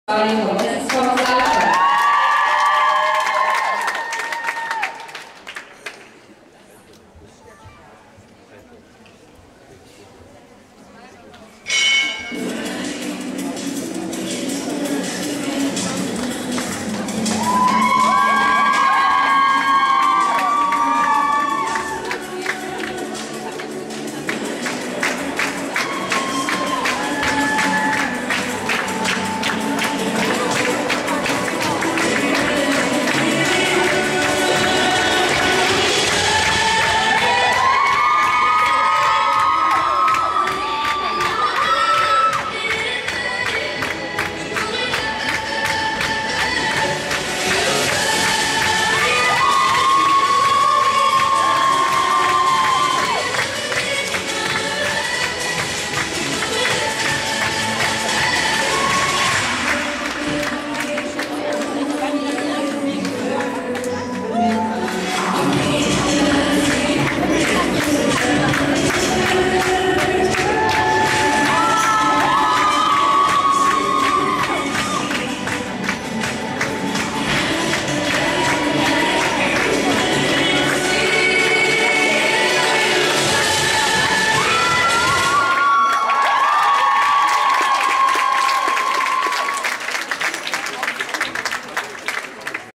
M2U02326
Aerobic Gymnastik WORLD CUP COMPETITION In Cantanhede Portugal.2023